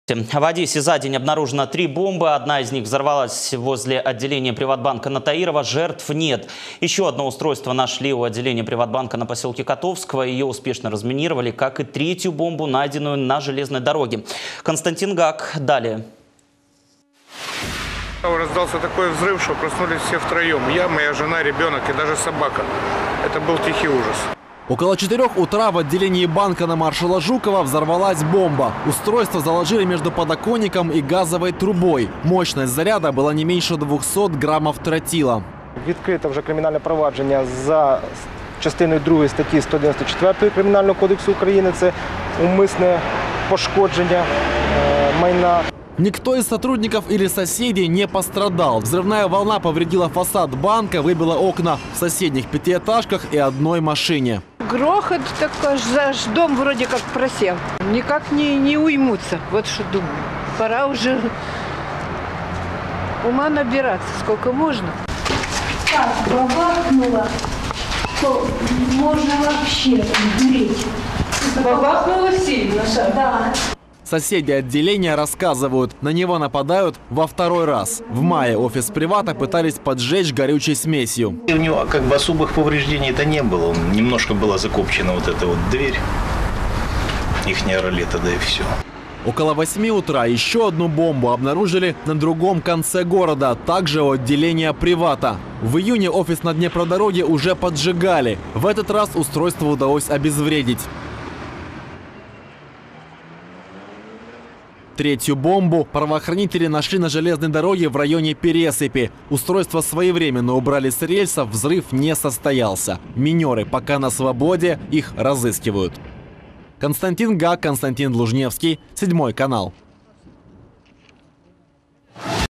В Одессе — три бомбы за день: одна взорвалась, две обезврежены
В Одессе за день обнаружено три бомбы. Одна из них взорвалась возле отделения «ПриватБанка» на Таирово. Жертв нет. Еще одно устройство нашли у отделения «Привата» на поселке Котовского — ее успешно разминировали, как и третью бомбу, найденную на железной дороге.